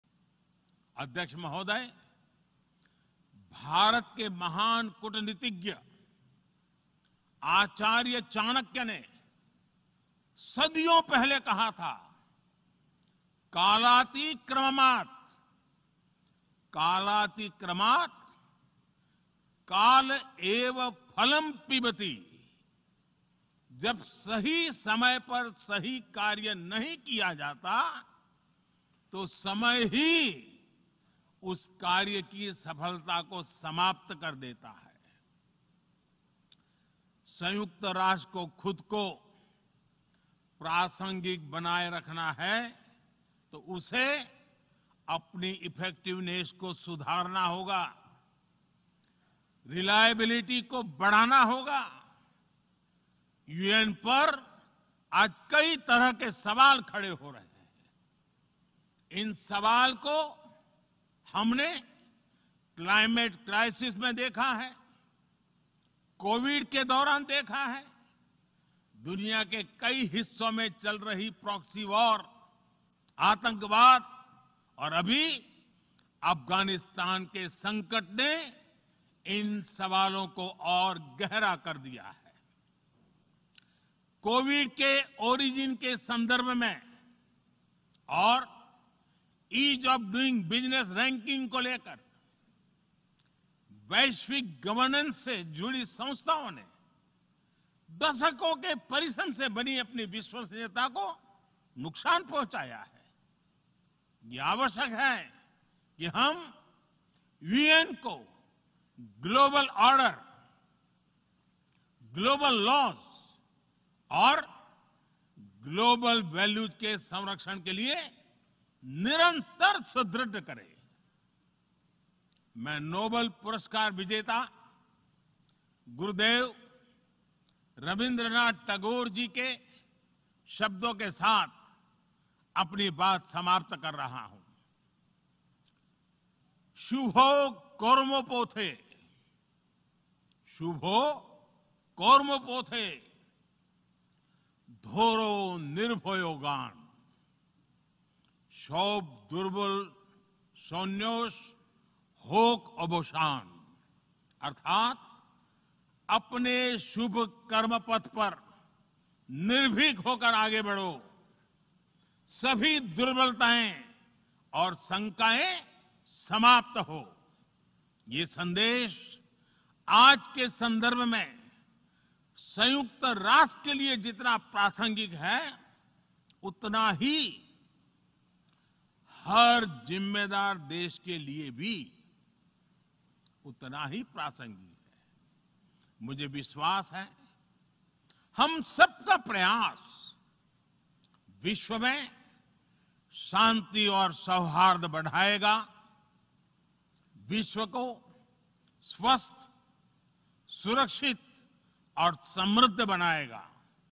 It is essential we constantly strengthen the UN to safeguard global order: PM
Prime Minister Narendra Modi's address to the United Nations General Assembly